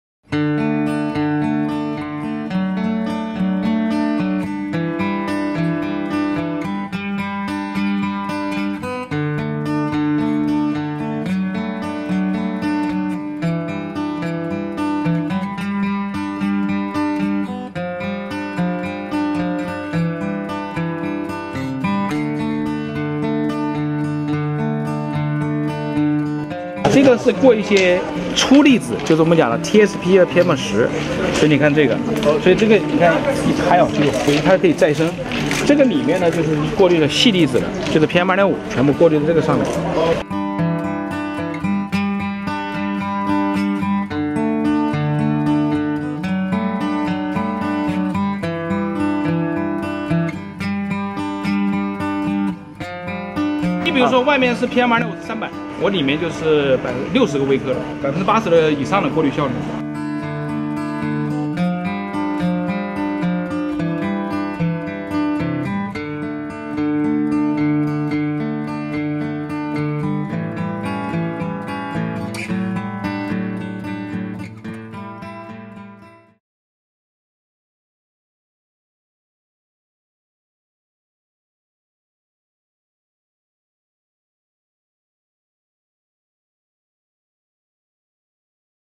World’s Largest Air Purifier Helps Reduce Haze in Northwest China City
An experimental 100-meter-high tower has been built in Xi'an City, northwest China's Shaanxi Province, in a bid to boost the fight against air pollution.

The chimney-like building does not pump out black smoke, but instead purifies the air, and has thus been dubbed the world's largest air purifier.